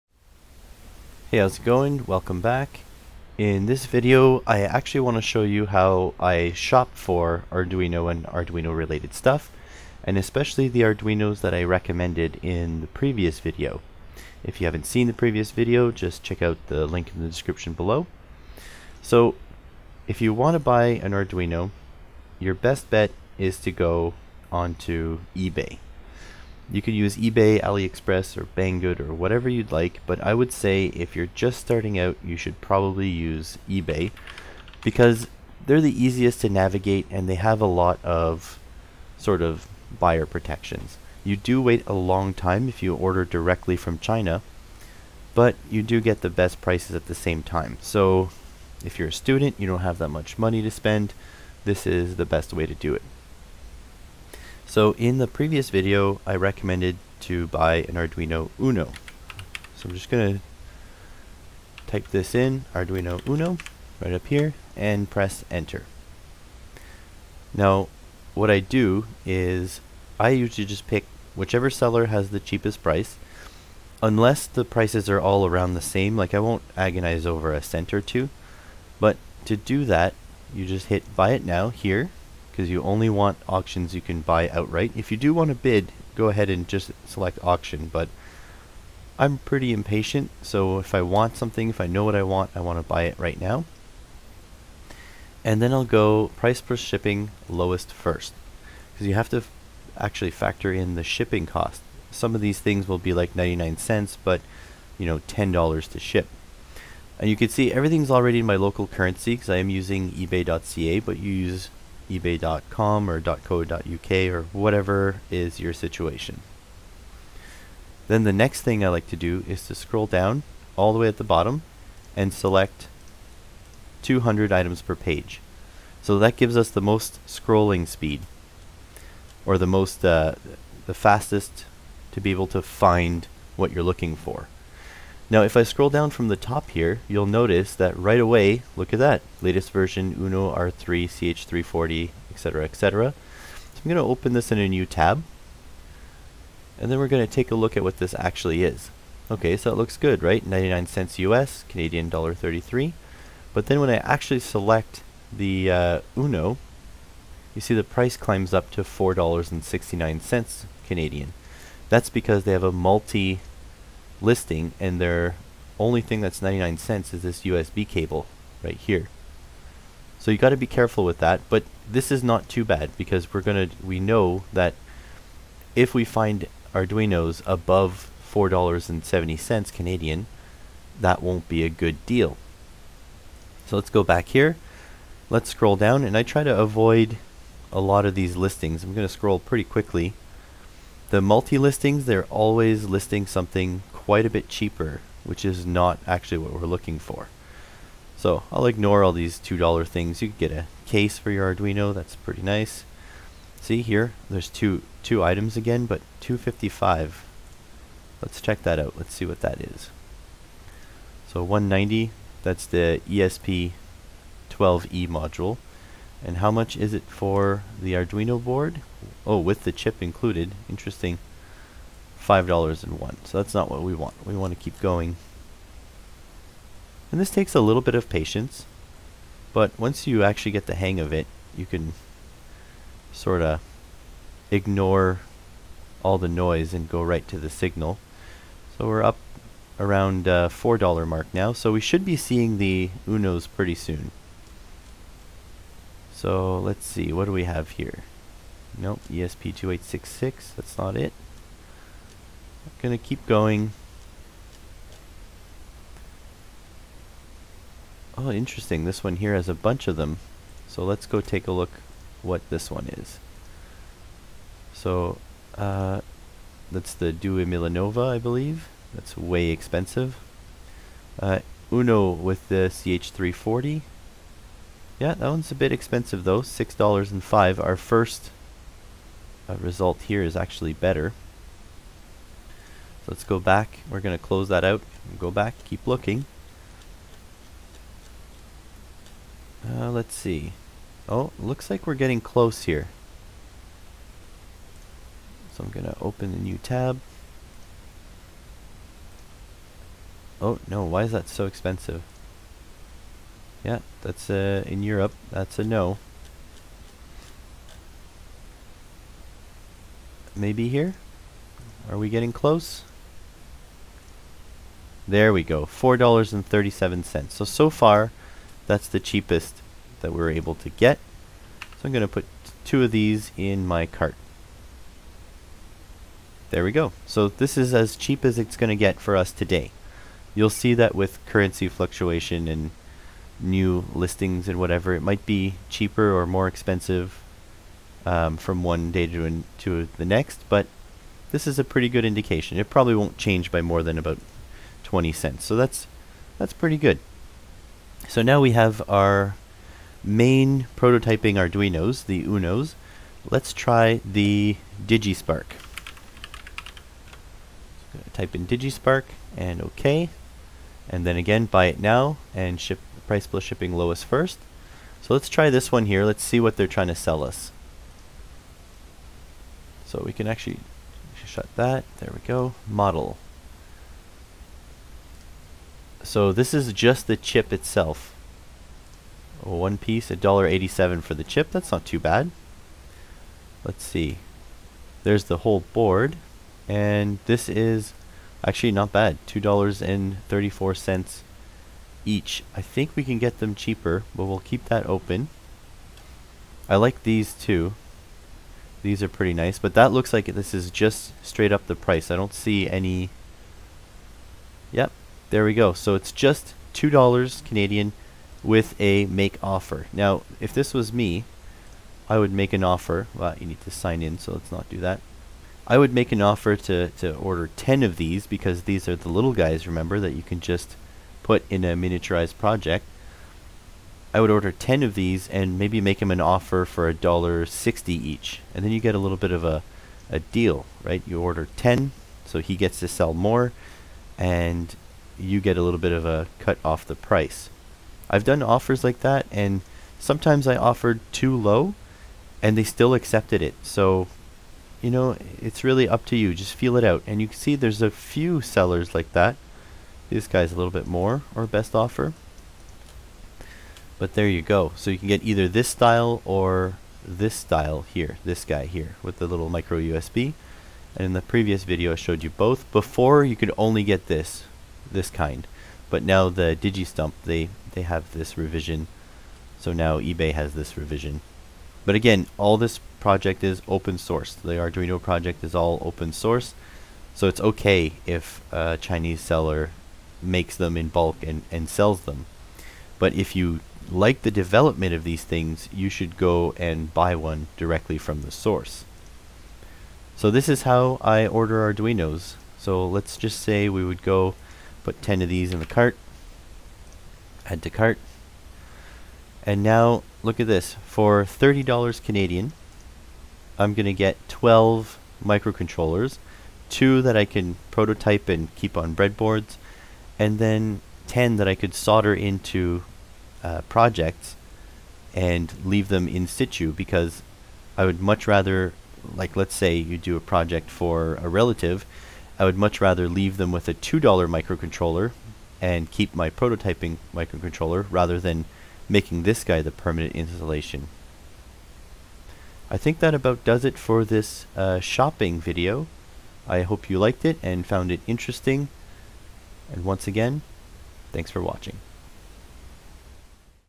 Arduino Basics - #2 - Shopping For Arduino
Part 2 of my Arduino Basics series!
In this video I'll go over how to get a great deal on an Arduino (non-official) board!